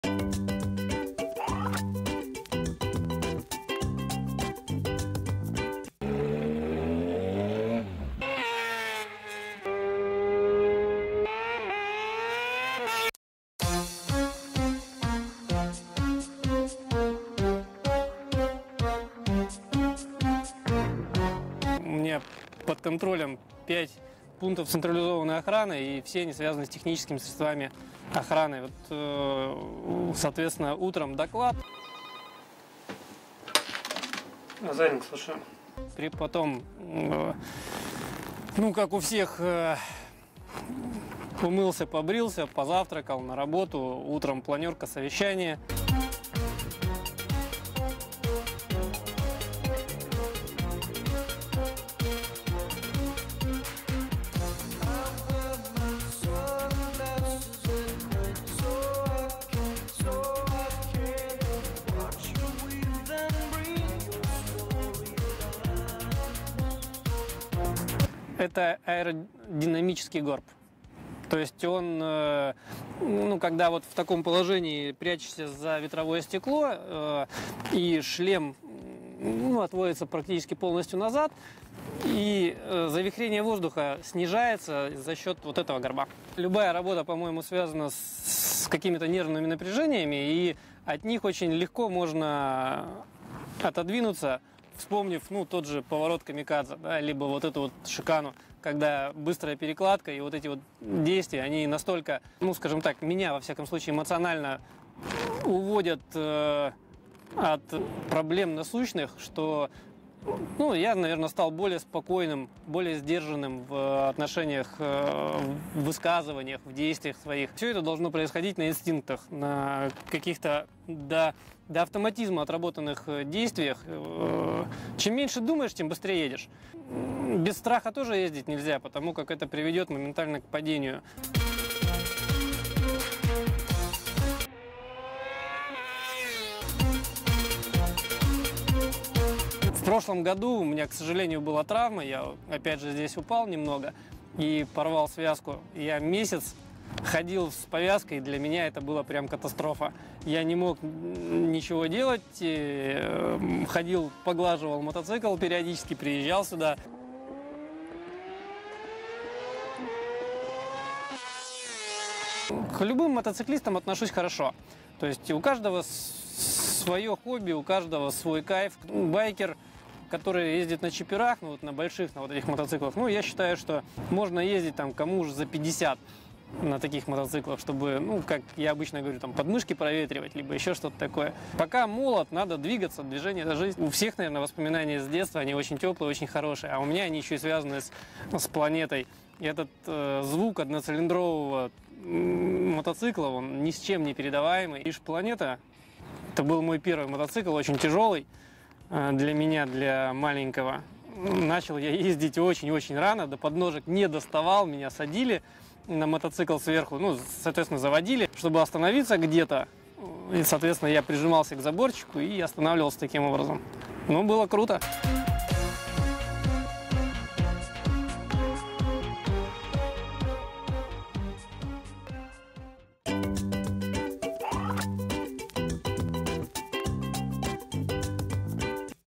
Горожане. Подполковник полиции и мотогонщик
На скорости свыше двухсот километров в час. Двухколёсное увлечение подполковника полиции: травмы и воспоминание о легендарном мотоцикле